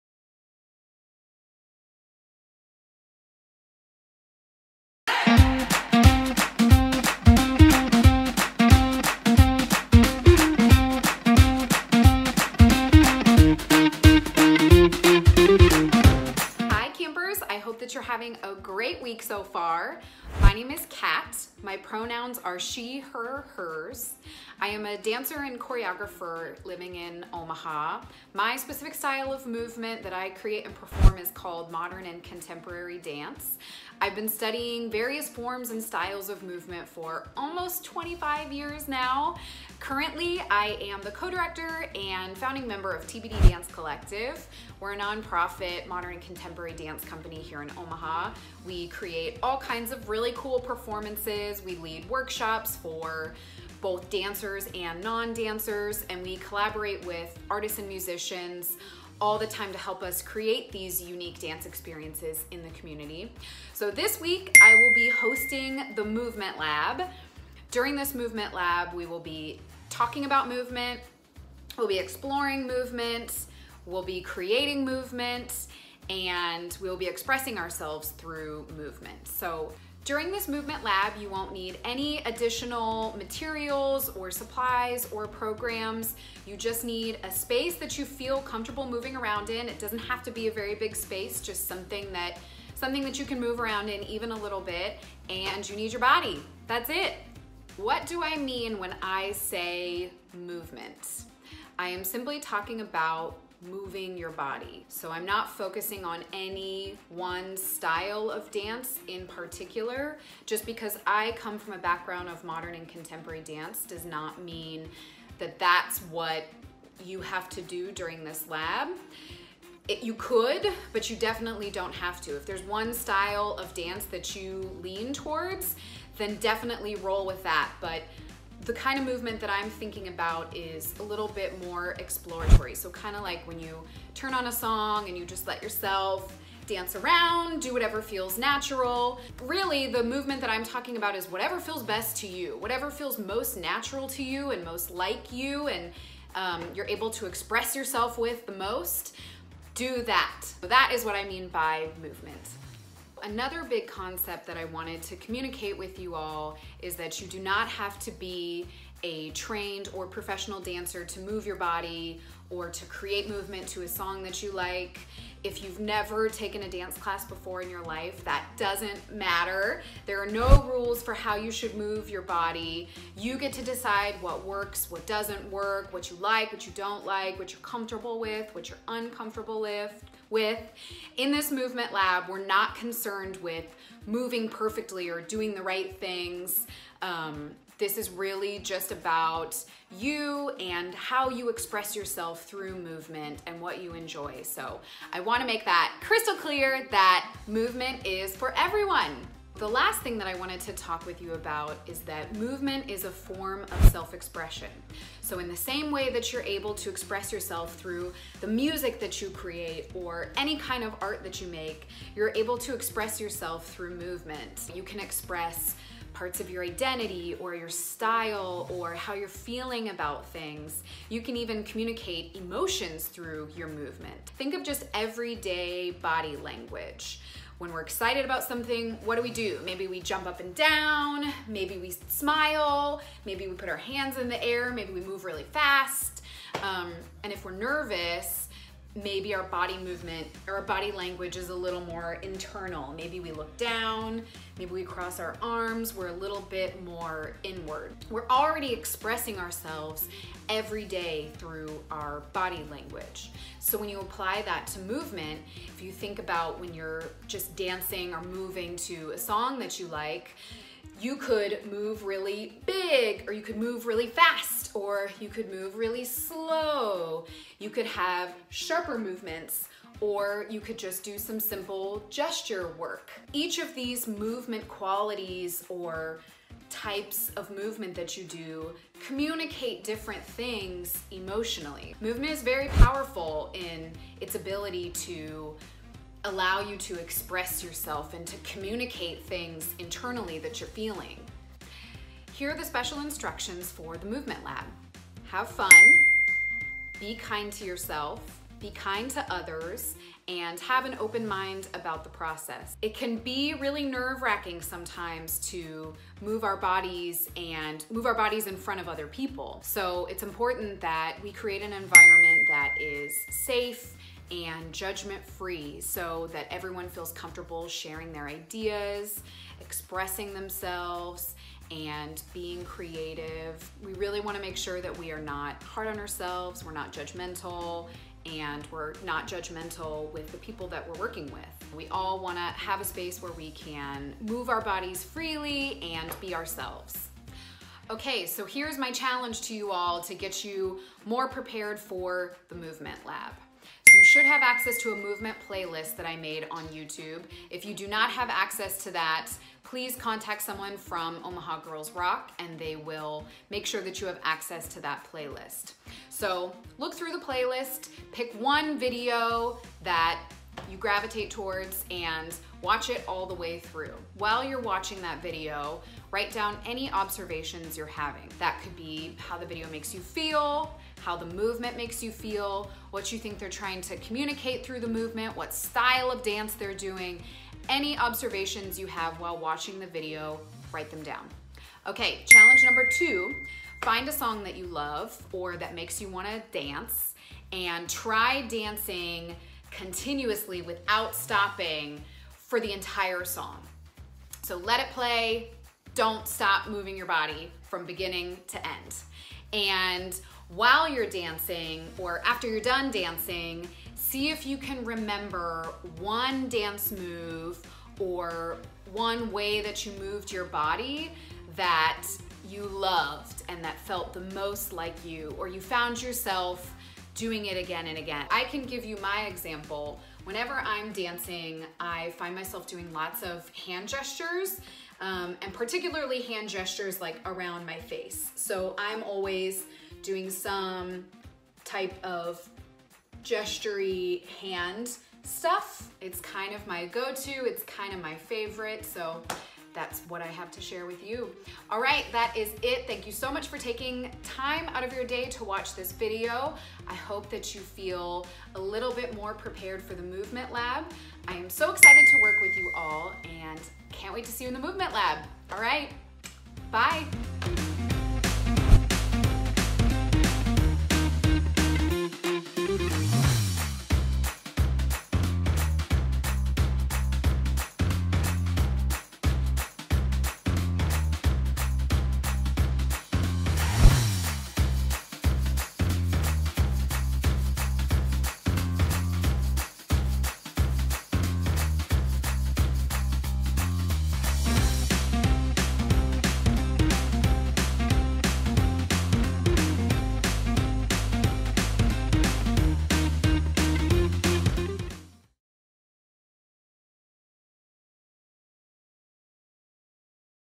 OurStory Project: Movement Study
Join tbd.dance collective co-founder, Kat Fackler, as she guides OGR teen artists in their second creation lab for the OurStory Project 2020.  Kat will explain herself, her work, movement as art and then give a mini-assignment to help make the live portion of the creation lab easier for all OGR artists.